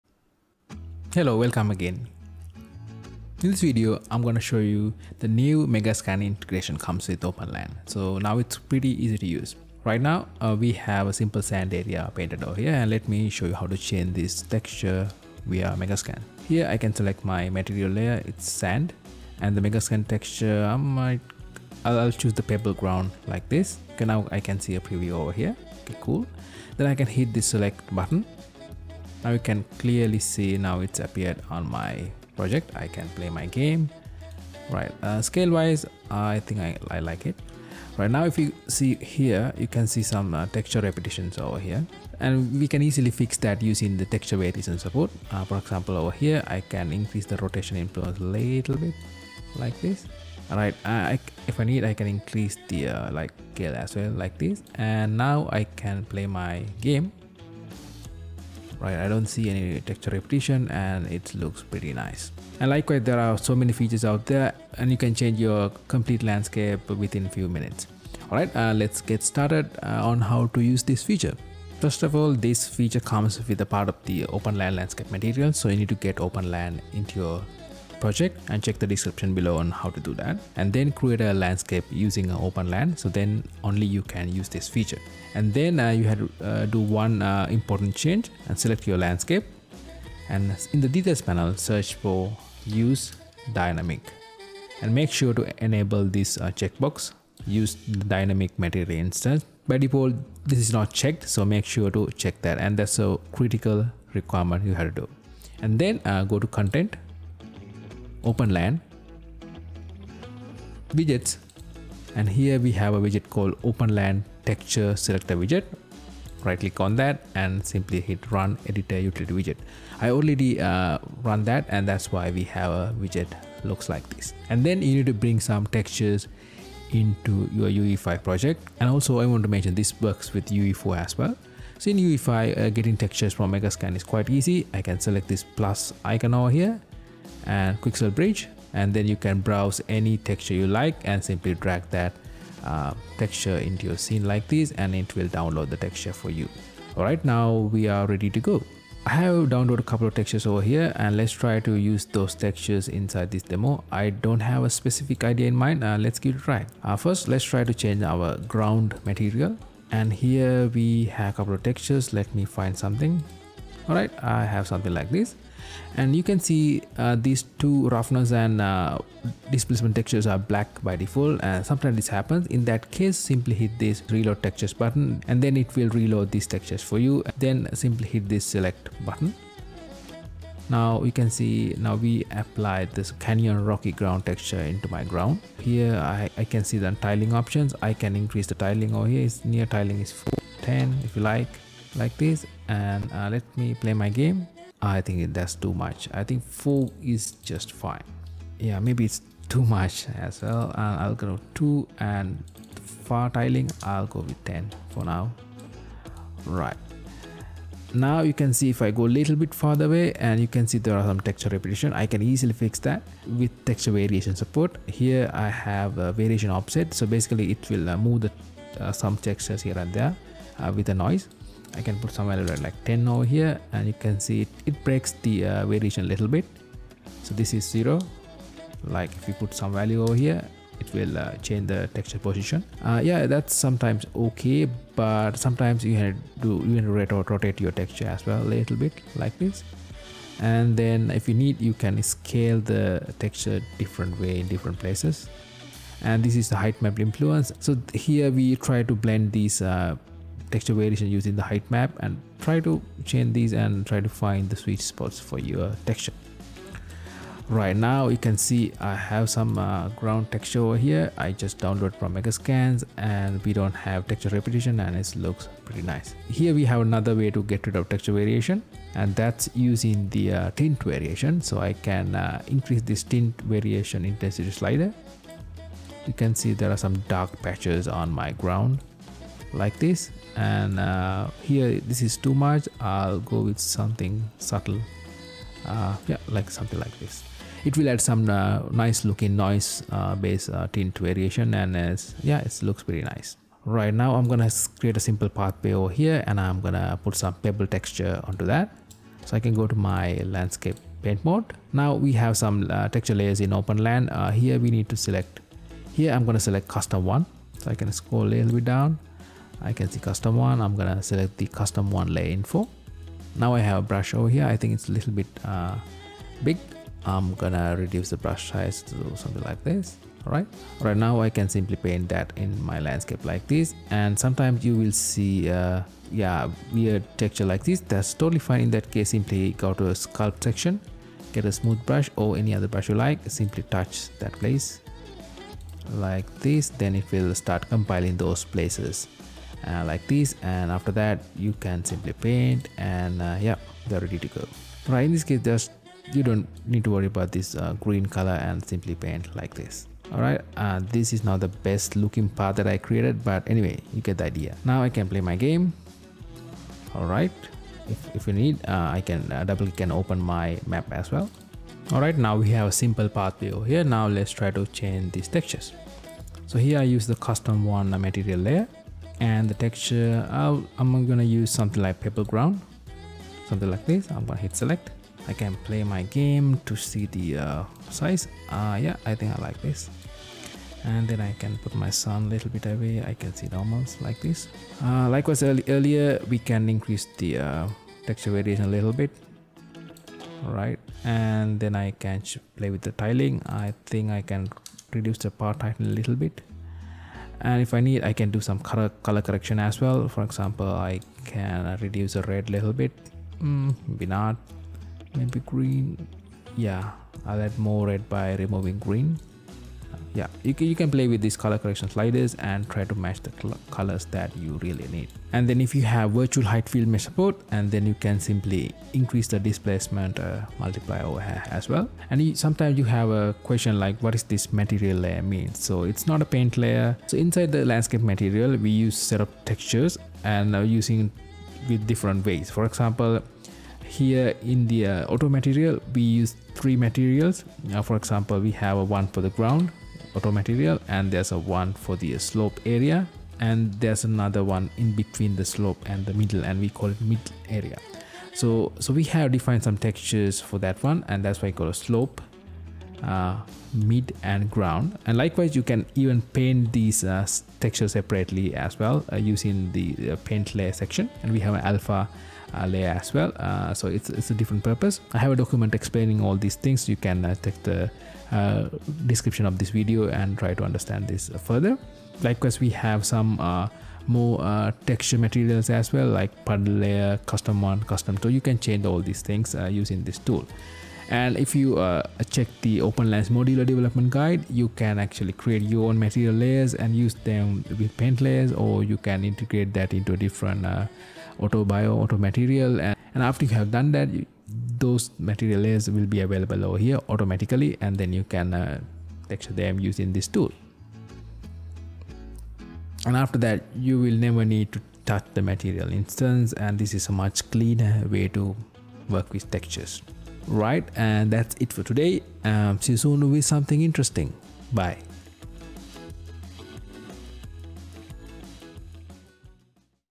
Use Megascans Textures for UE5 Landscapes Quickly with OpenLand
Now, OpenLand's Megascan Texture selection widget is very powerful & now you can control a lot of aspects of your material besides selecting textures. For example, now you can do texture variations & tint variations by simply changing a slider.

* 0:00 Intro/Demo
* 1:12 Get OpenLand
* 1:27 Enable Landscape Dynamic Material Instance
* 1:45 Open the Megascans Texture Seector Widget
* 2:12 Get Texures
* 2:34 Tutorial
* 7:00 OpenLand Material Layers